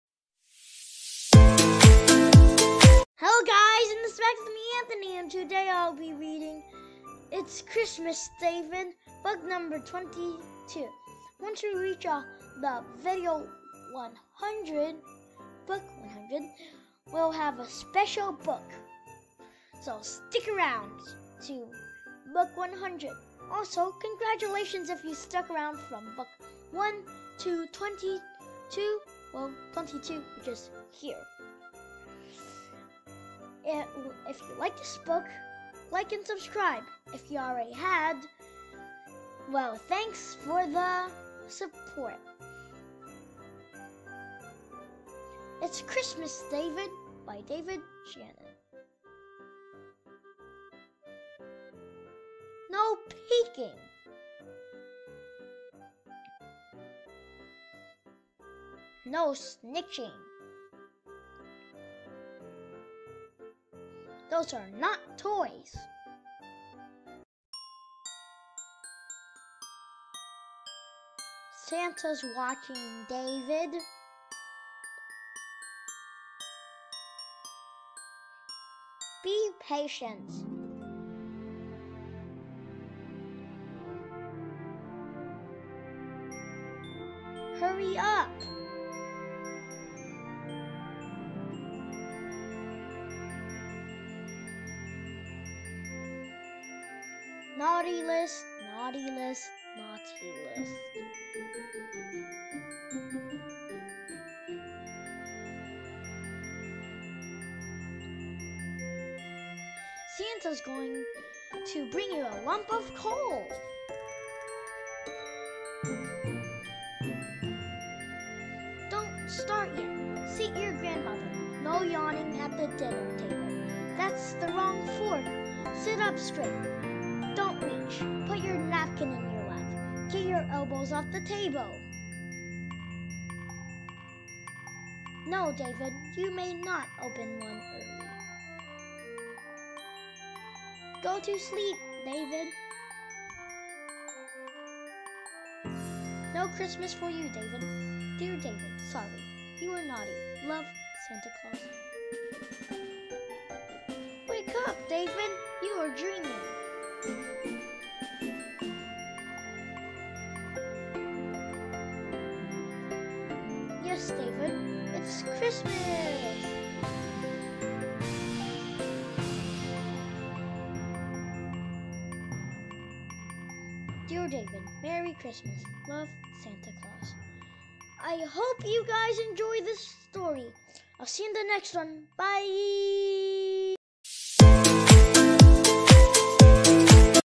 【英文故事绘本】🎄🎀🎀|It's Christmas David!| David Shannon| Story for Christmas
Happy holidays, Friends!

I'm going to read Christmas today, David! David Shannon. You may remember our friend David from No, David! He is always in trouble! Will he be on the good list in time for Christmas?

"NO, DAVID!'s best-selling star turns Christmas traditions upside down with hilarious humor.

Readers of all ages will vividly remember trying to sneak a peek at a hidden gift package; writing a wish list for Santa, and trying to be present at a formal Christmas dinner. Always in the background, we know Santa is watching, and soon it will be decided whether David deserves a shiny new fire truck or a piece of coal under a tree. From toying with delicate decorations to long lines for Santa, here are the usual Christmas activities - but also David's playful decorations. Destined to become the only way to become a classic of the year. "

00:00 welcome to my channel!
01:17 Santa's watching, David!
01:55 Sant's going to bring you a lump of coal!
02:44 Yes, it is Christmas!